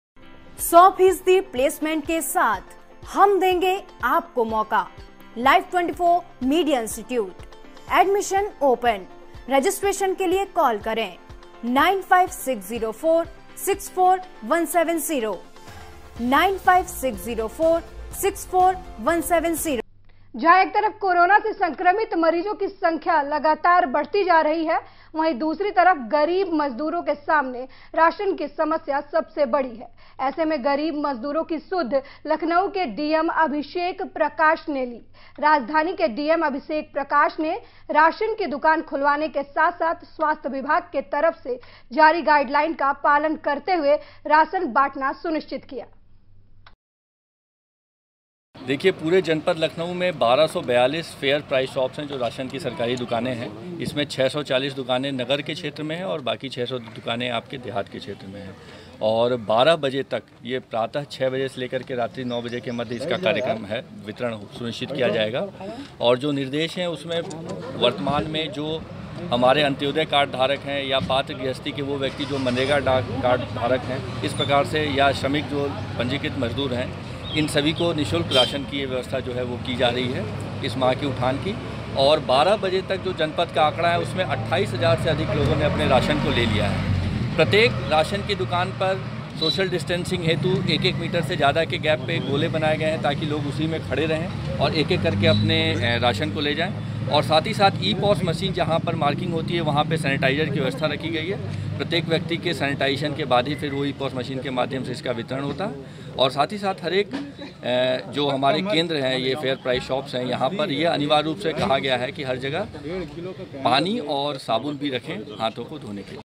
लखनऊ: गरीबों के लिए आगे आये DM अभिषेक प्रकाश, खुलवाईं दुकानें
LIVE24 NEWS